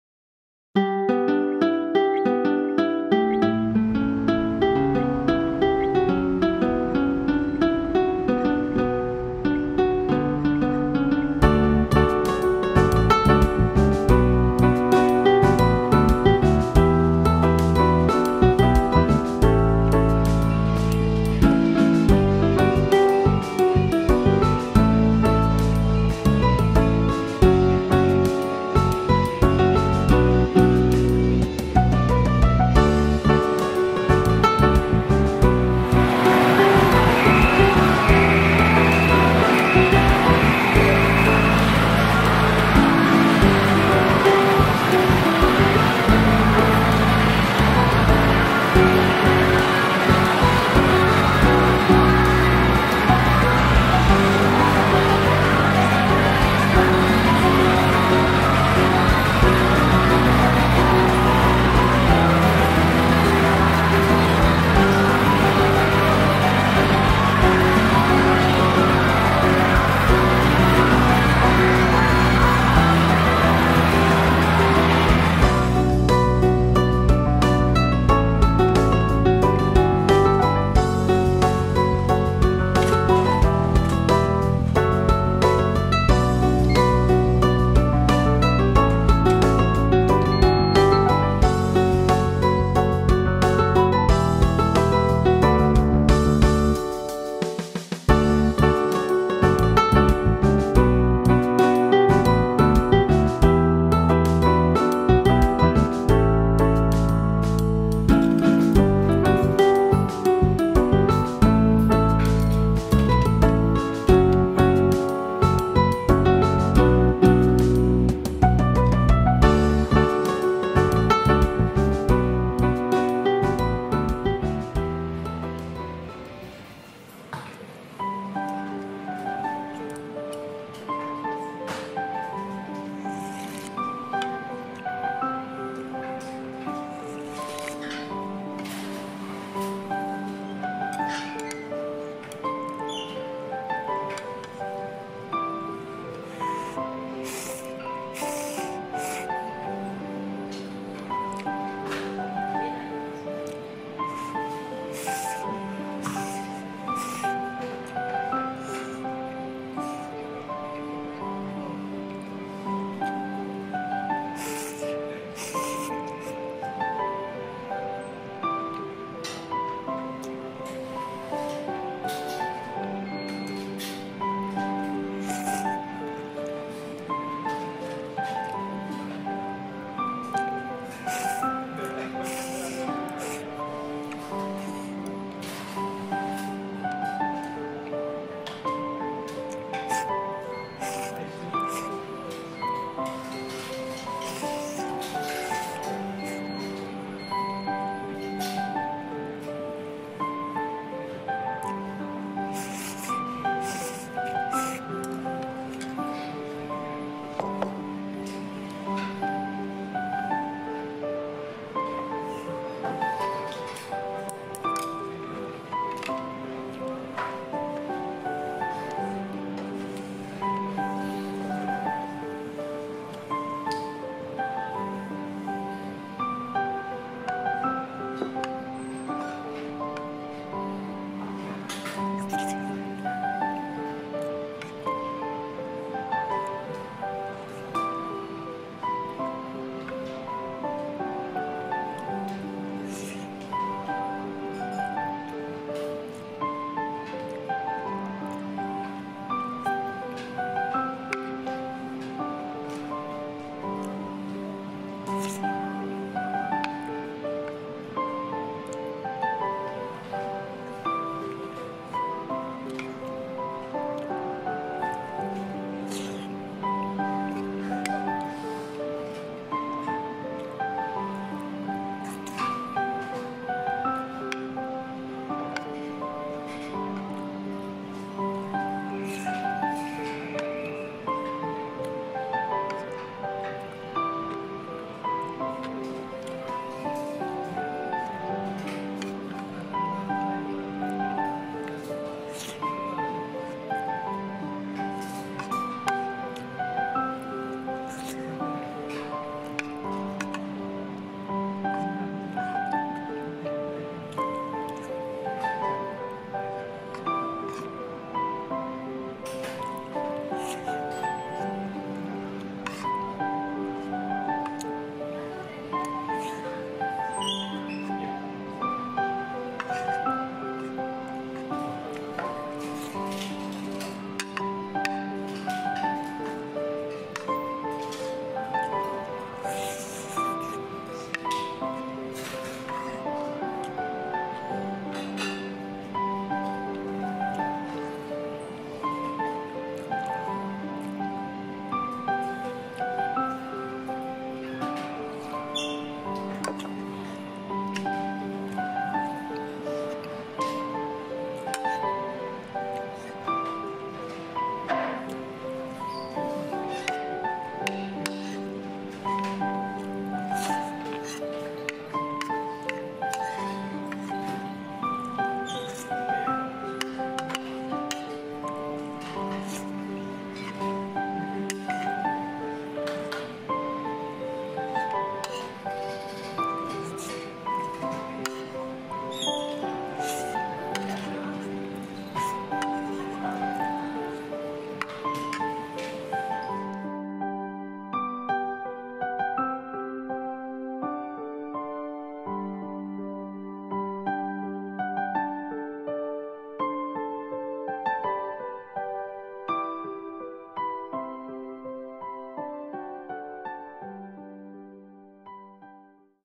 【勝どき食堂】 コスパ最高 カレーも醤油ラーメンも290円を食す ＃695
勝どき食堂　浜野店看板が気になり行って来ました